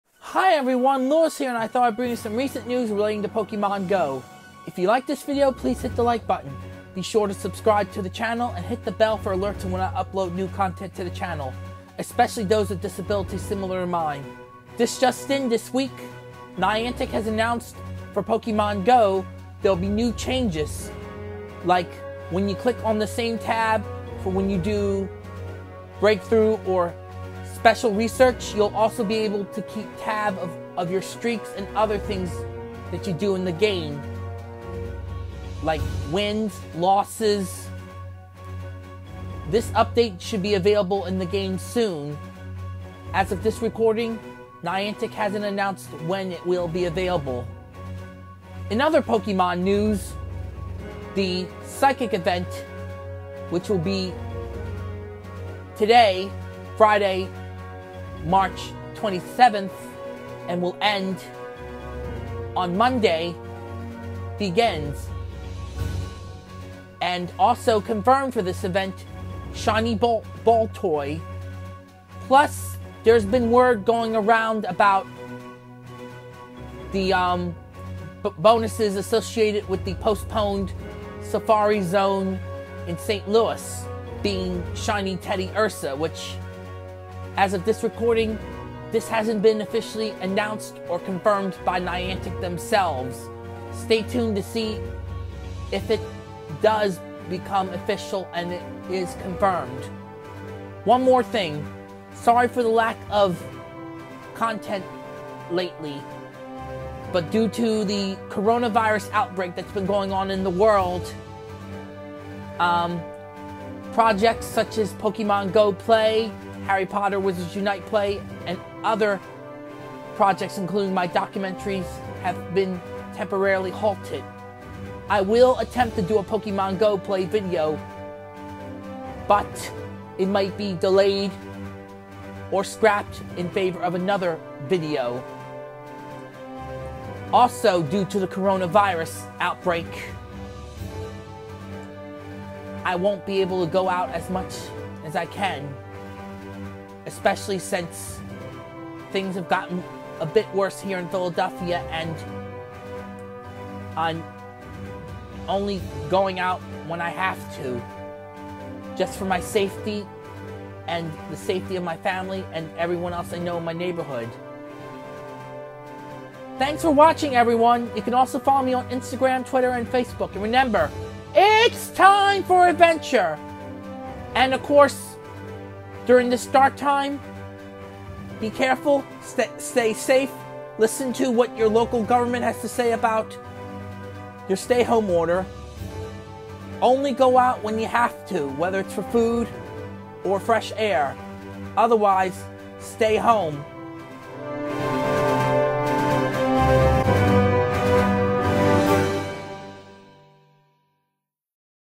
Pokémon Go News Episode 145 (Psychic Pokémon Event)
In this video I talk about the psychic Pokémon event in the game. Also shiny Baltoy is now in the game everyone along with new Psychic type Pokémon from Unova.